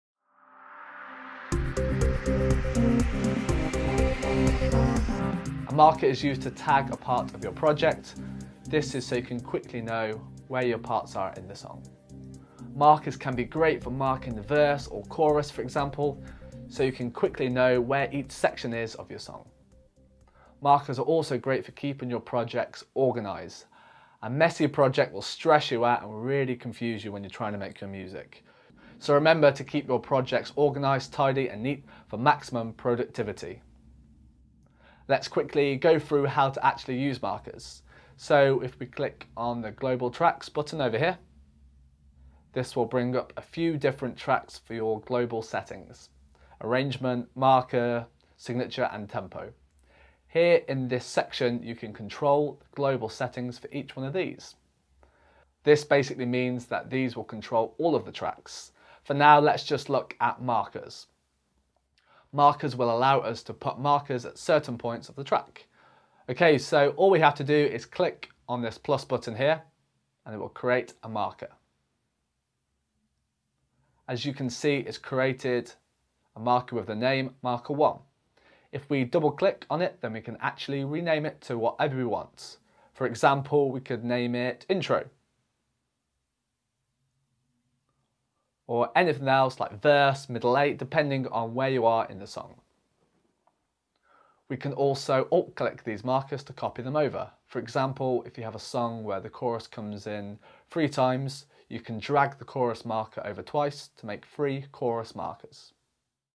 How to use Markers in Logic Pro X
In this video, I teach you how to use the markers in Logic Pro X

Cheers,
Tomas